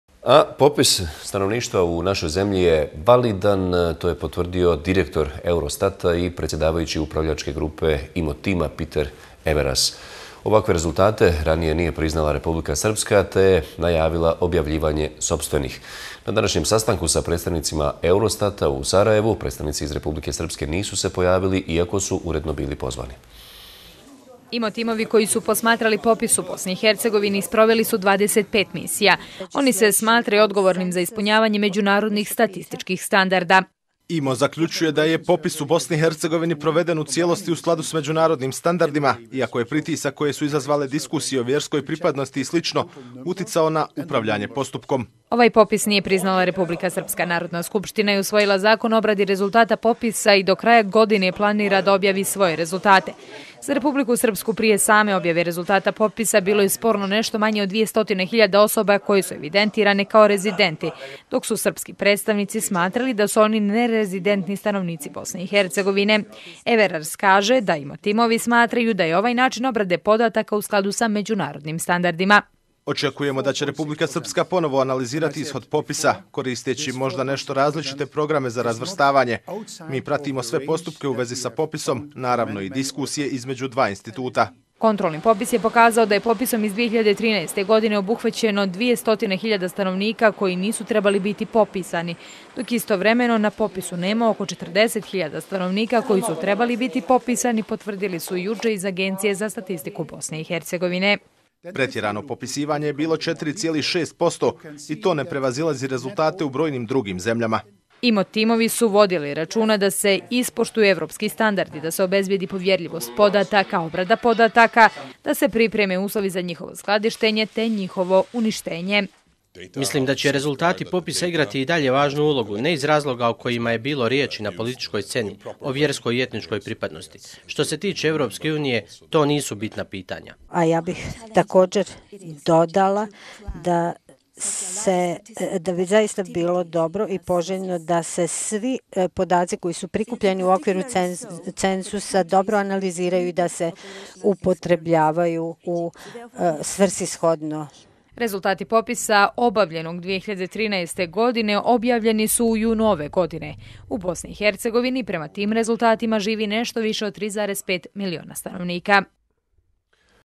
Eurostat potvrdio - Popis stanovništva validan
Popis stanovništva u BiH je validan, to je zvanično danas sapoštio direktor EUROSTAT-a i predsjedavajući Upravljačke grupe IMO-tima Peter Everaers. Ovakve rezultate popisa ranije nije priznala RS, te je najavila objavljivanje sopstvenih rezultata.
Na današnjem sastanku sa predstavnivim EUROSTAT-a u Sarajevu nisu se pojavili predstavnici iz RS, iako su bili pozvani  zajedno sa predstavnicima Agencije za statistiku BiH,  i federalnog Zavoda.

IMO- timovi, koji su posmatrali popis u BiH sproveli su 25 misija. Oni se smatraju odgovornim da li su ispunjeni međunarodni statistički standardi.

"IMO zaključuje da je popis u BiH proveden u cjelosti u skladu sa međunarodnim standardima, iako je pritisak koje su izazvale diskusije o vjerskoj pripadnosti i slično uticao na upravljanje postupkom“, kaže direktor EUROSTAT-a Pieter Everars.

Ovaj popis nije priznala RS. Narodna Skupština je usvojila zakon o obradi rezultata popisa, i do kraja godine planira da objavi svoje rezultate. Za RS prije same objave popisa bilo je sporno nešto manje od 200 hiljada osoba koje su evidentirane kao rezidenti, dok su srpski predstavnici smatrali da su oni nerezidentni stanovnici BiH. Everars kaže da Imo timovi smatraju da je ovaj način obrade podataka u skladu sa međunarodnim standardima.

"Očekujemo da će RS ponovo analizirati ishod popisa. Koristeći možda nešto različite programe za razvrstavanje. Mi pratimo sve postupke u vezi sa popisom naravno i diskusije između dva instituta“, kaže Everars.

Kontrolni popis je pokazao da je popisom iz 2013. obuhvaćeno je 200.000 stanovnika BiH koji nisu trebali biti popisani, dok istovremeno na popisu nema oko 40.000 stanovnika koji su trebali biti popisani potvrdili su juče u Agenciji za statistiku BIH.

"Pretjerano popisivanje je bilo 4,6%, i to ne prevazilazi reuzltate u brojnim drugim zemljama“, pojašnjava direktor EUROSTAT-a.

IMO timovi su vodili računa da se ispoštuju evropski standardi, da se obezbijedi povjerljivost podataka, obradi podataka, da se pripreme uslovi za njihovo skladištenje te njihovo uništenje.

"Mislim da će rezultati popisa igrati i dalje važnu ulogu ne iz razloga o kojima je bilo riječi na političkoj sceni, o vjerskoj i etničkoj pripadnosti. Što se tiče EU to nisu bitna pitanja“, smatra Lars Gunar Vigemar, šef delegacije EU u BiH.

"Ja bih takođe dodala da da bi svi podaci koji su prikupljeni u okviru cenzusa dobro analiziraju i da se upotrebljavaju svrsishodno“, kaže predstavnik IMO tima u UN Keiko Osaki Tomita.

Rezultati popisa obavljenog 2013. godine objavljeni su u junu ove godine. U BiH prema tim rezultatima živi nešto više od 3,5 miliona stanovnika.